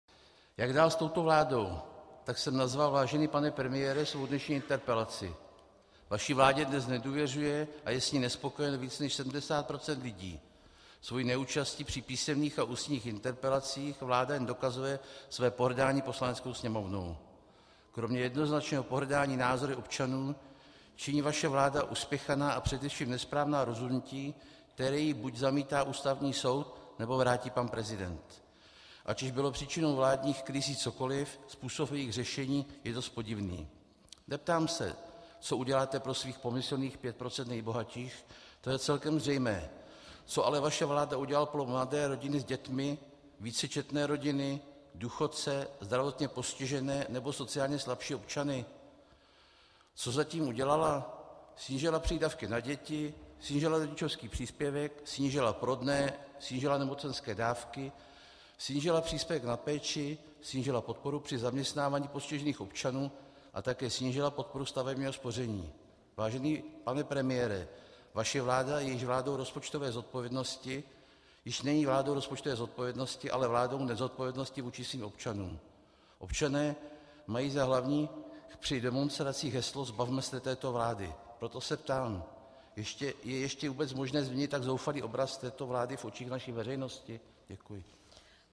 Jan Látka: Nečasova vláda nezodpovědnosti vůči občanům
Jan Látka, poslanec za ČSSD - záznam řeči v Poslanecké sněmovně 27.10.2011

Jak dál s touto vládou? Tak jsem nazval, vážený pane premiére, svou dnešní interpelaci. Vaší vládě dnes nedůvěřuje a je s ní nespokojeno více než 70 % lidí. Svojí neúčastí při písemných a ústních interpelacích vláda jen dokazuje své pohrdání Poslaneckou sněmovnou. Kromě jednoznačného pohrdání názory občanů činí vaše vláda uspěchaná a především nesprávná rozhodnutí, která jí buď zamítá Ústavní soud, nebo vrátí pan prezident.

Ať už bylo příčinou vládních krizí cokoliv, způsob jejich řešení je dost podivný. Neptám se, co uděláte pro svých pomyslných pět procent nejbohatších. To je celkem zřejmé. Co ale vaše vláda udělala pro mladé rodiny s dětmi, vícečetné rodiny, důchodce, zdravotně postižené nebo sociální slabší občany? Co zatím udělala? Snížila přídavky na děti, snížila rodičovský příspěvek, snížila porodné, snížila nemocenské dávky, snížila příspěvek na péči, snížila podporu při zaměstnávání postižených občanů a také snížila podporu stavebního spoření.

Vážený pane premiére, vaše vláda již není vládou rozpočtové zodpovědnosti, ale vládou nezodpovědnosti vůči svým občanům. Občané mají za hlavní při demonstracích heslo "Zbavme se této vlády!". Proto se ptám: Je ještě vůbec možné změnit tak zoufalý obraz této vlády v očích naší veřejnosti?

Děkuji.